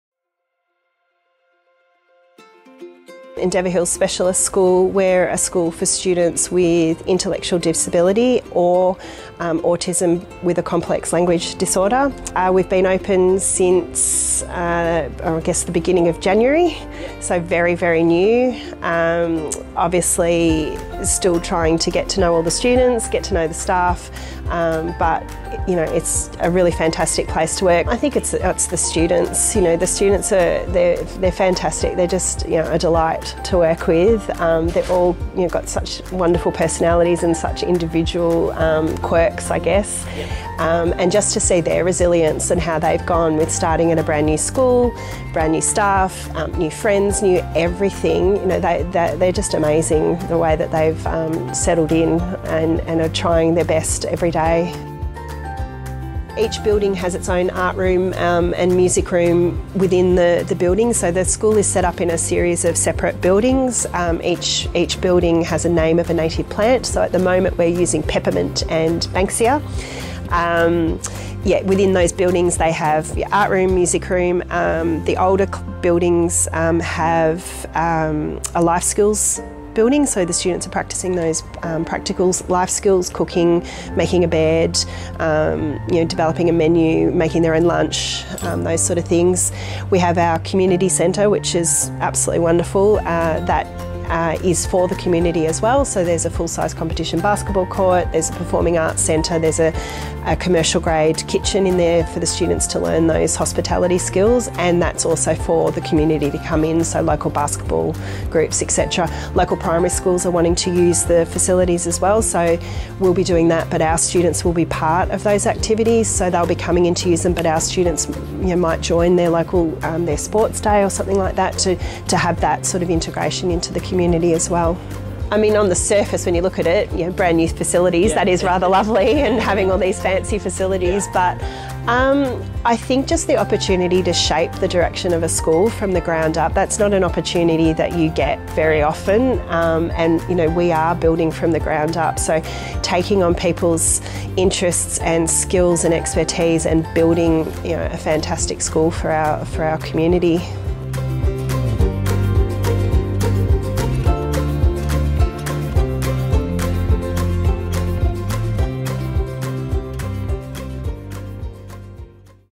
Endeavour Hills Specialist School
Say hello to the brand new Endeavour Hills Specialist School, located in Melbourne's South East! With incredible facilities and friendly educators and students, this school is looking to expand its 2022 cohort in the next coming months.